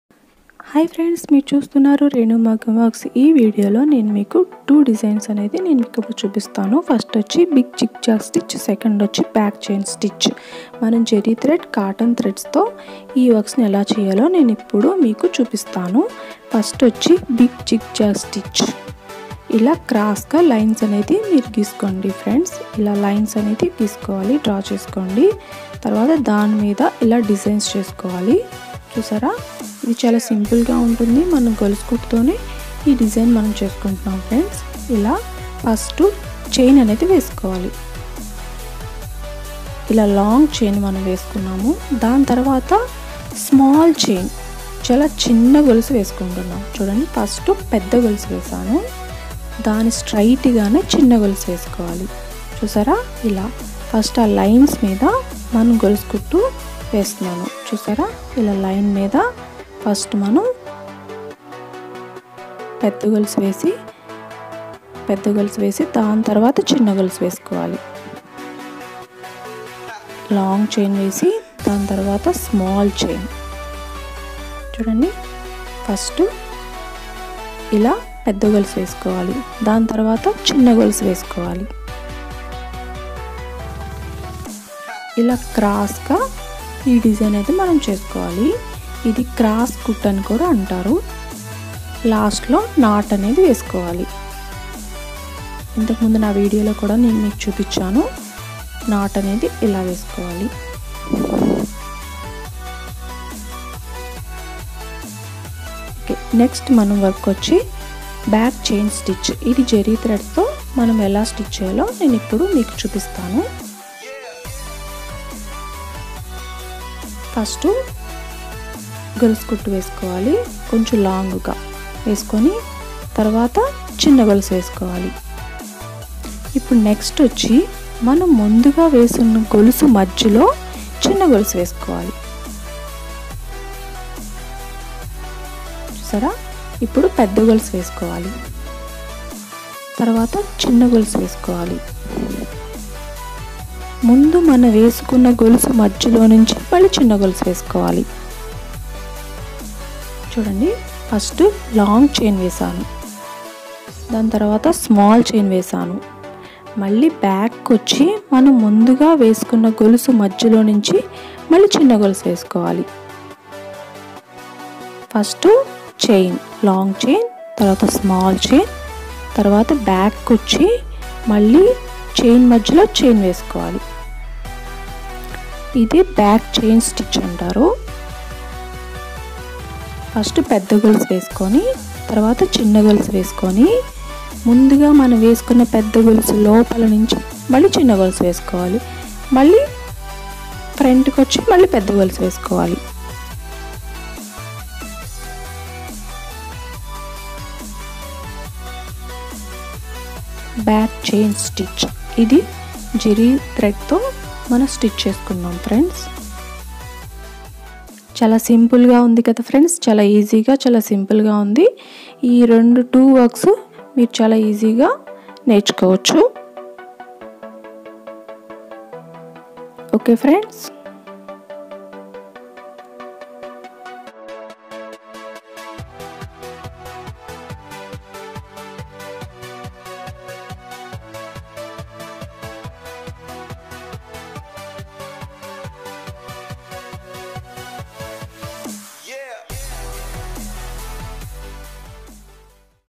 Big Zig Zag stitch and back chain stitch in telugu | క్రాస్ కుట్టు and back chain stitch తెలుగులో
IN THIS TUTORIAL VIDEO YOU ARE GOING TO KNOW ABOUT